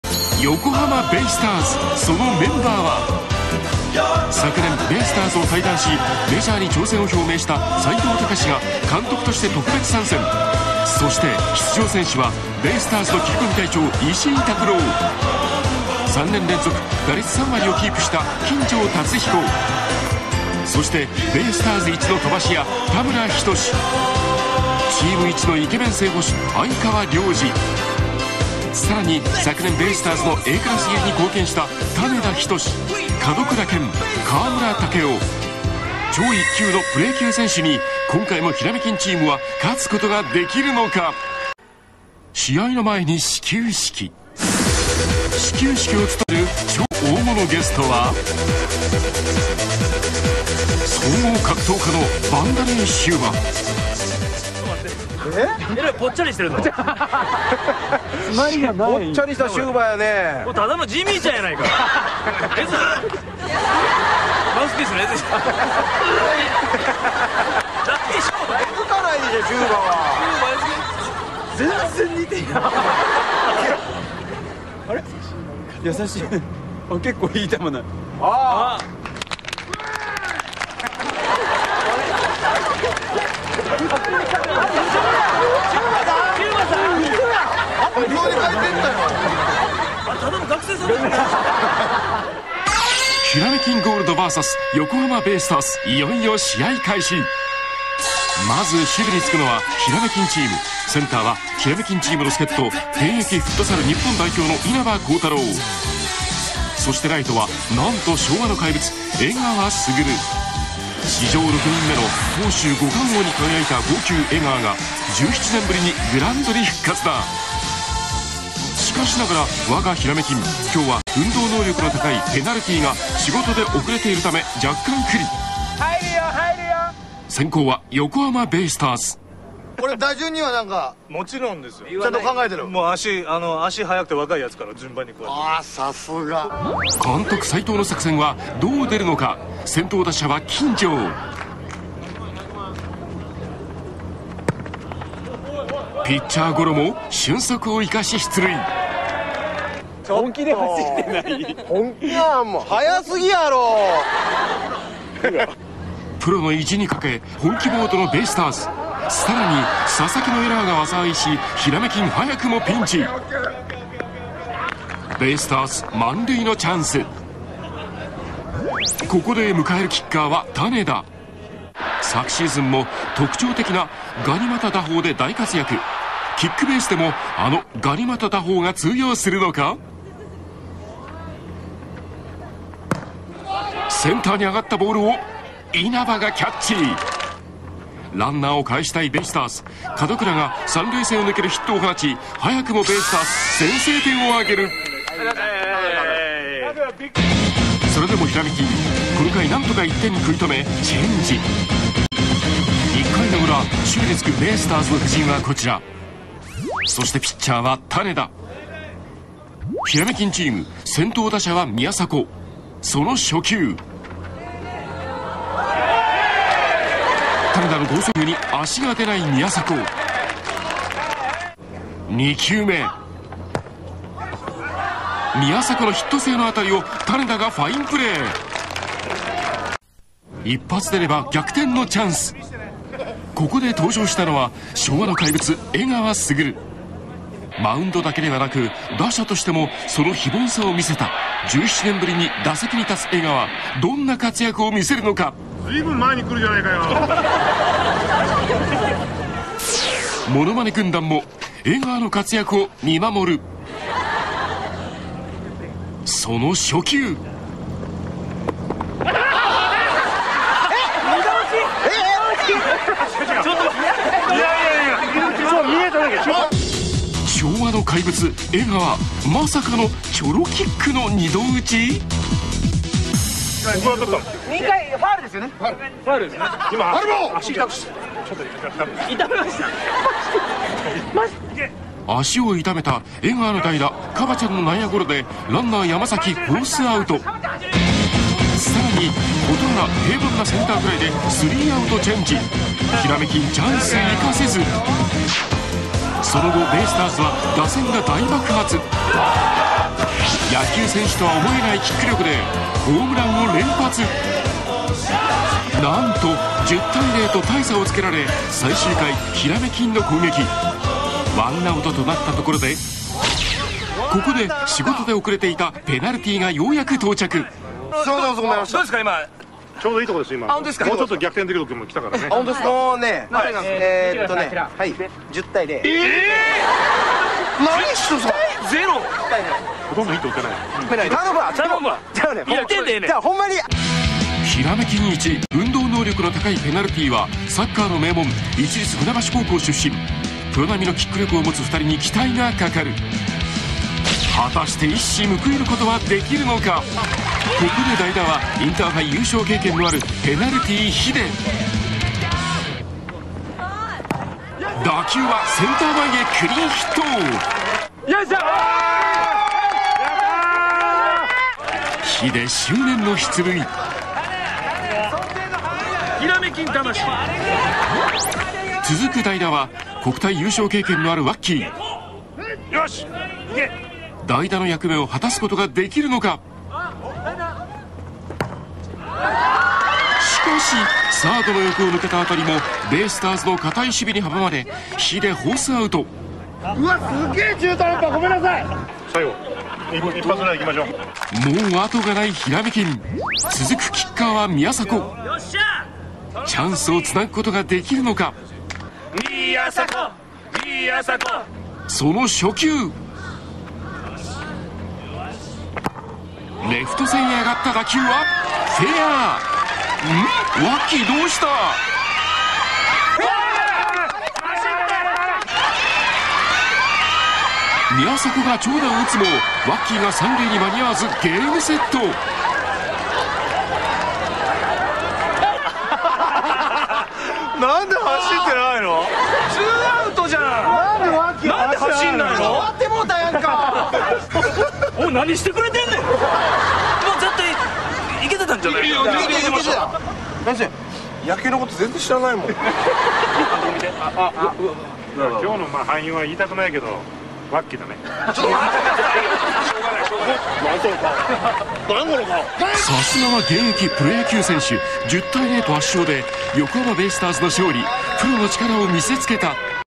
ベイスターズの選手とキックベース
斎藤隆、石井琢朗、相川亮二がコーチとして帰ってきたので。
2005年オフ

金城龍彦、多村仁志、種田仁、川村丈夫、門倉健